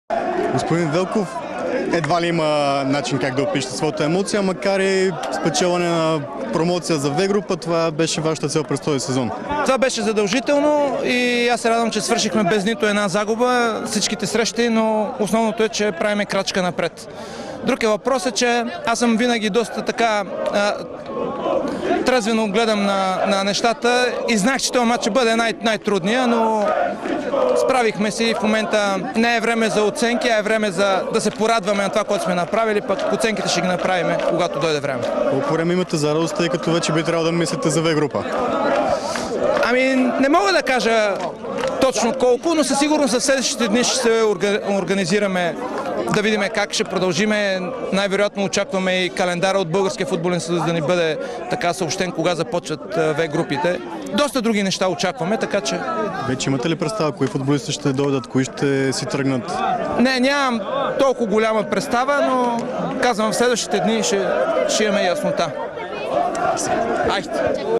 Антон Велков: Радвам се, че не допуснахме нито една загуба през сезона
Антон Велков остана доволен от представянето на Локомотив през сезона.